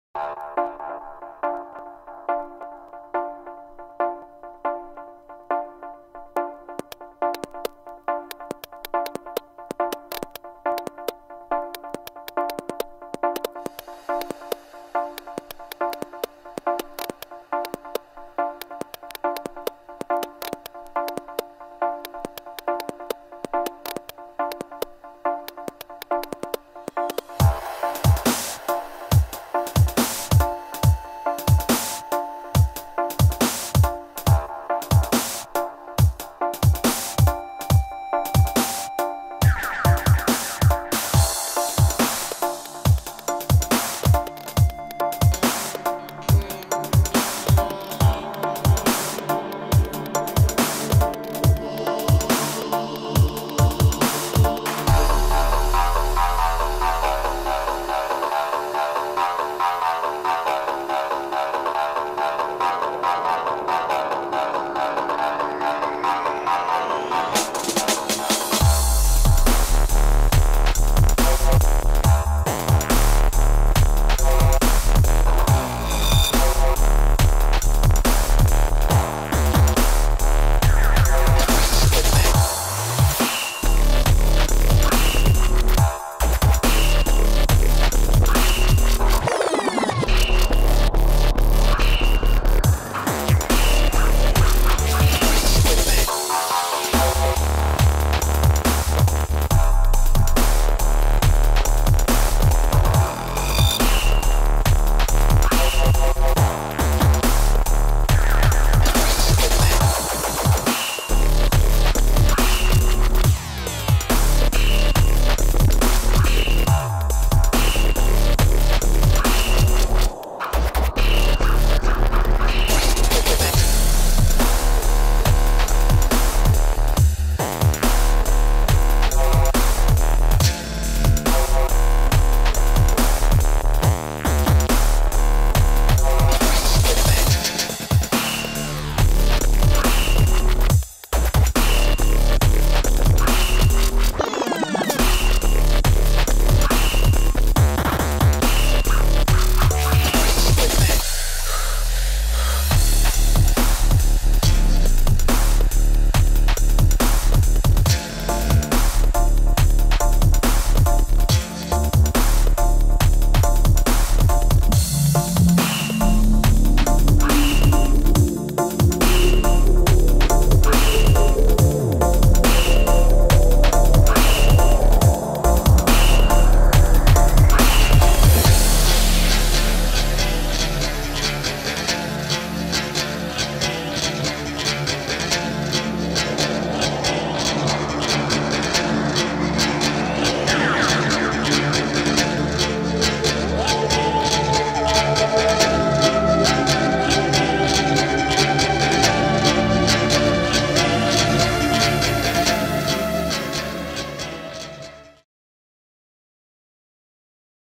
14 days of airbrush in 3 minutes!
Watch Daftpixel in action as these production hours turn white canvas into a masterpiece in a quick entertaining time lapse.